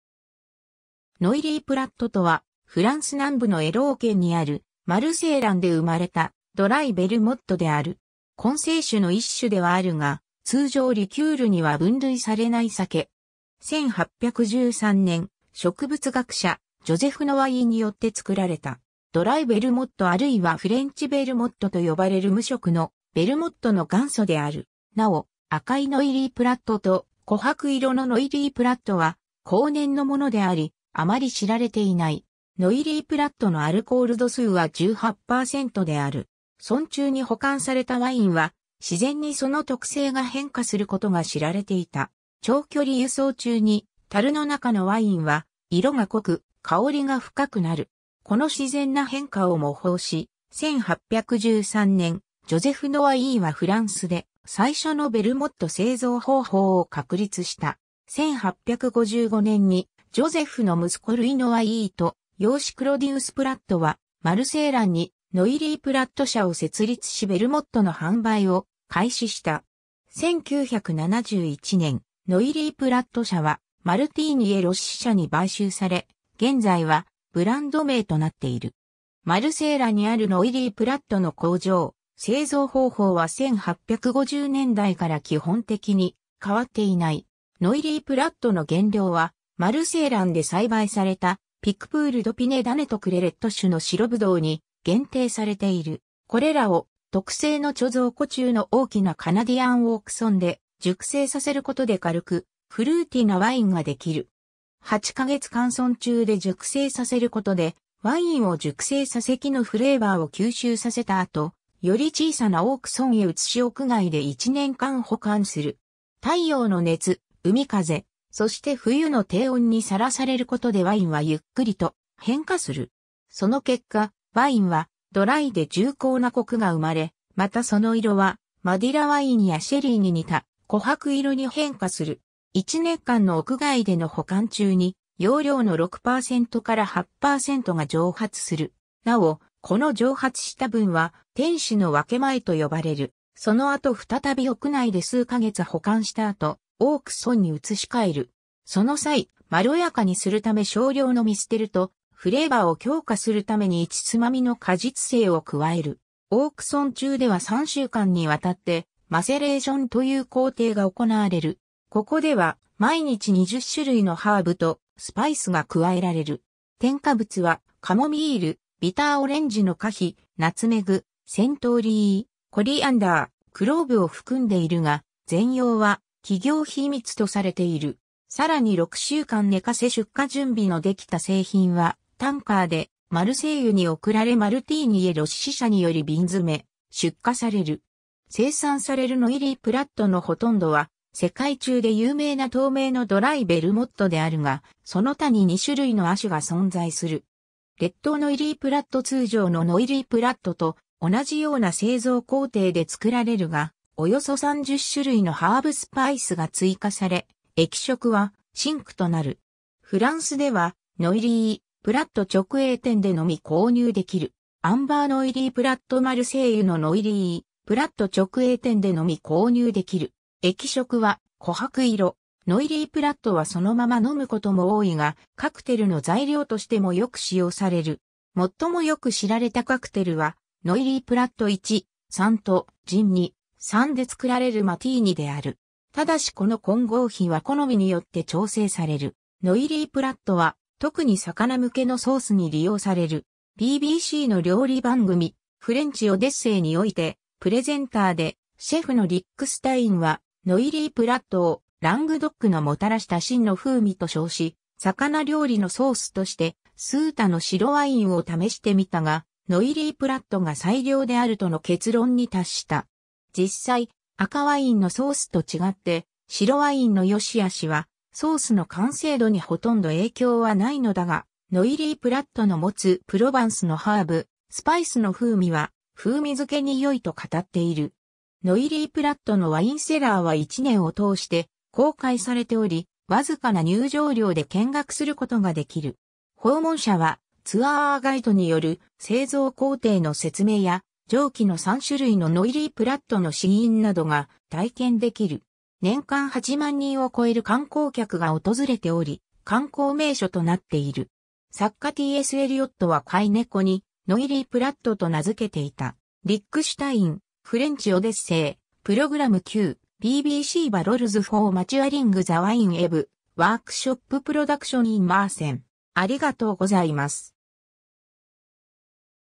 ノイリー・プラット
ノイリー・プラット(Noilly Prat, フランス語ではノワイイ･プラット)とは、フランス南部のエロー県にあるマルセイラン(Marseillan)で生まれたドライ・ベルモットである。
 混成酒の1種ではあるが、通常リキュールには分類されない酒。
 1813年、植物学者ジョゼフ・ノワイイ(Joseph Noilly)によって作られた。
 ドライ・ベルモットあるいはフレンチ・ベルモットと呼ばれる無色のベルモットの元祖である。
 なお、赤いノイリープラットと琥珀色のノイリープラットは後年のものであり、あまり知られていない。
 ノイリー・プラットのアルコール度数は18%である。
 樽中に保管されたワインは、自然にその特性が変化することが知られていた。
 長距離輸送中に樽の中のワインは色が濃く香りが深くなる。
 この自然な変化を模倣し、1813年ジョゼフ・ノワイイはフランスで最初のベルモット製造方法を確立した。
 1855年に、ジョゼフの息子ルイ・ノワイイ(Louis Noilly)と養子クロディウス・プラット(Claudius Prat)はマルセイランに ノイリー・プラット社を設立しベルモットの販売を開始した。
 1971年，ノイリー・プラット社はマルティーニ・エ・ロッシ社に買収され、現在はブランド名となっている。
 マルセイランにあるノイリー・プラットの工場 製造方法は1850年代から基本的に変わっていない。
 ノイリー・プラットの原料はマルセイランで栽培された、ピクプール・ド・ピネ種とクレレット種の白ブドウに限定されている。
 これらを特製の貯蔵庫中の大きなカナディアンオーク樽で熟成させることで軽くフルーティなワインができる。
 8か月間樽中で熟成させることで、ワインを熟成させ木のフレーバーを吸収させた後、より小さなオーク樽（バレル）へ移し屋外で一年間保管する。
 太陽の熱、海風、そして冬の低温にさらされることでワインはゆっくりと変化する。
 その結果、ワインはドライで重厚なコクが生まれ、またその色はマディラ・ワインやシェリー (ワイン)に似た琥珀色に変化する。
 1年間の屋外での保管中に、容量の6%～8%が蒸発する。
なお、この蒸発した分は、天使の分け前（Angel's share）と呼ばれる。
 その後再び屋内で数か月保管した後、オーク樽（カスク）に移し替える。
 その際、まろやかにするため少量のミステル（ブドウ果汁と蒸留酒）と、フレーバーを強化するために一つまみの果実精を加える。
 オーク樽中では3週間にわたって（おそらくノイリー・プラット特有の）マセレーション（浸漬）という工程が行われる。
 ここでは毎日20種類のハーブとスパイスが加えられる。
添加物はカモミール、ビターオレンジの果皮、ナツメグ、セントーリー、コリアンダー、クローブを含んでいるが、全容は企業秘密とされている。
 さらに6週間寝かせ出荷準備のできた製品は、タンカーでマルセイユに送られマルティーニ・エ・ロッシ社により瓶詰・出荷される。
 生産されるノイリー・プラットのほとんどは、世界中で有名な透明のドライ・ベルモットであるが、その他に2種類の亜種が存在する。
 レッドノイリー・プラット 通常のノイリー・プラットと同じような製造工程で作られるが、およそ30種類のハーブ・スパイスが追加され、液色は深紅となる。
フランスではノイリー・プラット直営店でのみ購入できる。
 アンバーノイリー・プラット マルセイユのノイリー・プラット直営店でのみ購入できる。
液色は琥珀色。
 ノイリー・プラットはそのまま飲むことも多いが、カクテルの材料としてもよく使用される。
 最も良く知られたカクテルは、ノイリープラット 1/3とジン 2/3 で作られるマティーニである。
ただしこの混合比は好みによって調整される。
 ノイリー・プラットは、特に魚向けのソースに利用される。
 BBCの料理番組「フレンチ・オデッセイ」(French Odyssey)において、プレゼンターでシェフのリック・スタイン（Rick Stein）はノイリー・プラットを「ラングドックのもたらした真の風味」と称し，「魚料理のソースとして数多の白ワインを試してみたが、ノイリー・プラットが最良であるとの結論に達した。実際、赤ワインのソースと違って、白ワインの良し悪しはソースの完成度にほとんど影響はないのだが、ノイリー・プラットの持つプロヴァンスのハーブ、スパイスの風味は風味づけに良い」と語っている。
 ノイリー・プラットのワイン・セラーは...